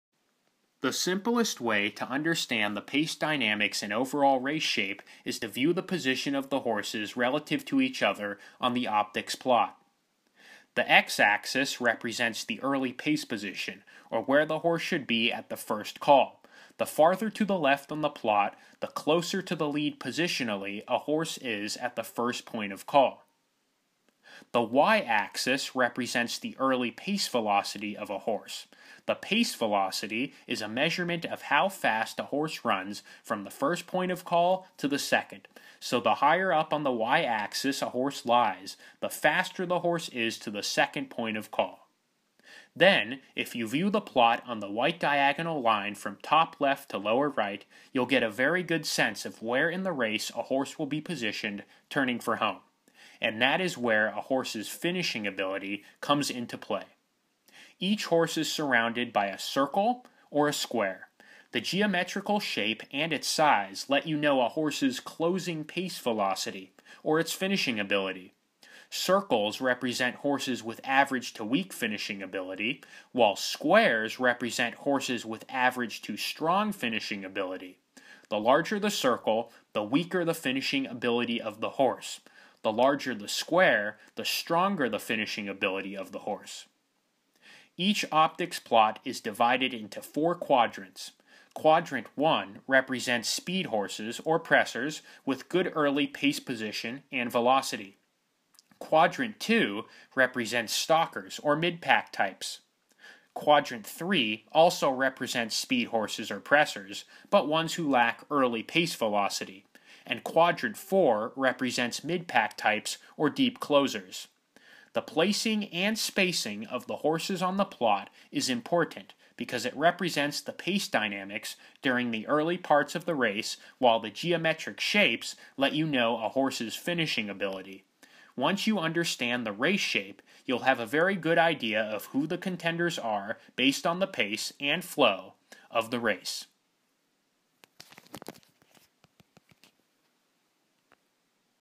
OptixPLOT basics
The basic OptixPLOT quadrant is explained